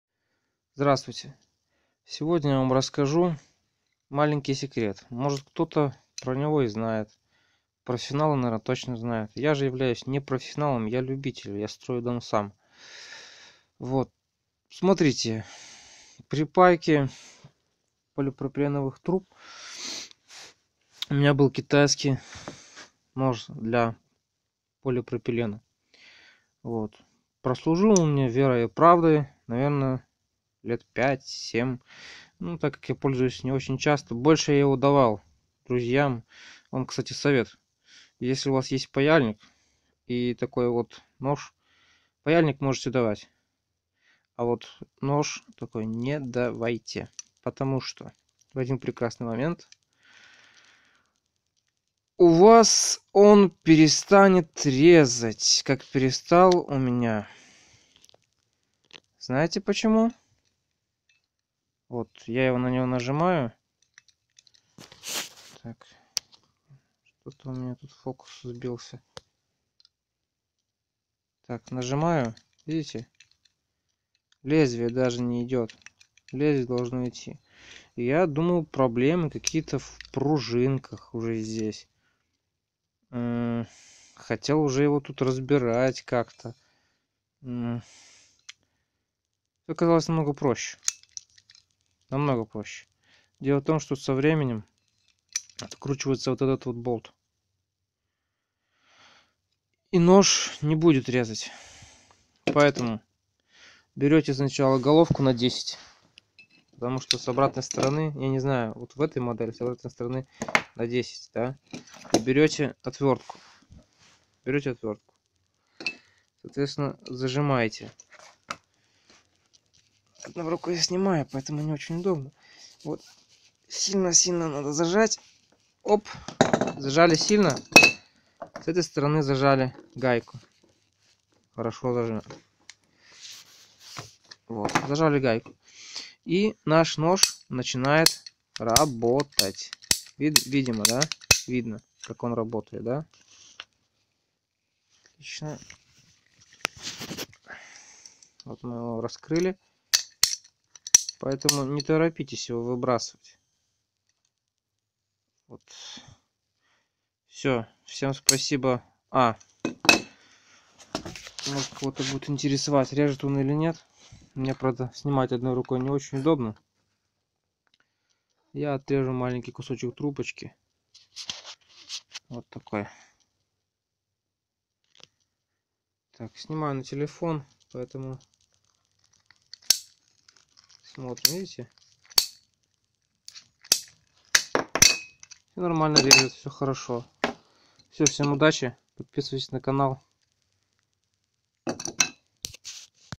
Ремонт ножниц для полипропиленовых труб
Сломались ножницы(нож) для полипропилена?не спешите выкидывать,может ещЁ не всЁ потеряно.